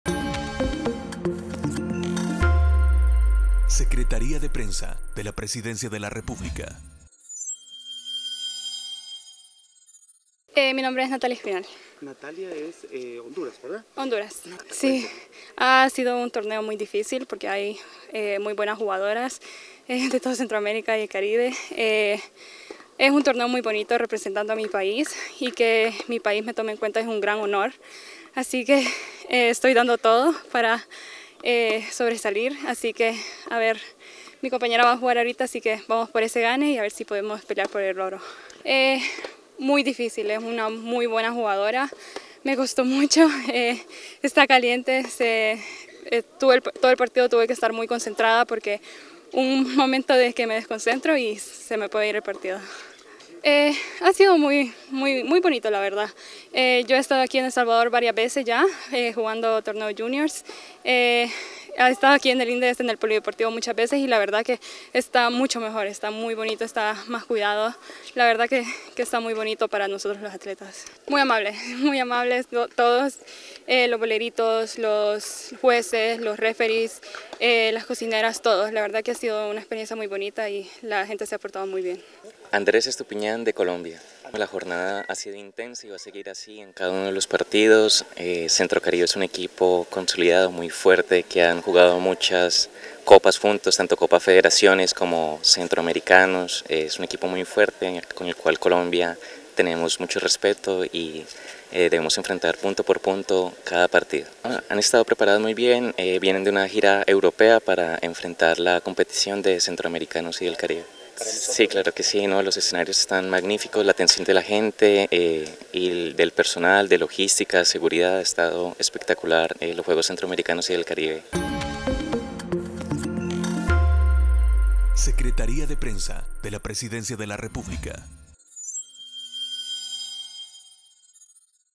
Colombia y México irán busca del oro de los XXIV Juegos Centroamericanos y del Caribe
SanSalvador2023 | Este viernes se han programado las finales entre Colombia y México en la Copa de Naciones categoría femenina, quienes disputarán el oro. En tanto, las tenistas de Centro Caribe Sports y Honduras se enfrentarán para alcanzar el bronce.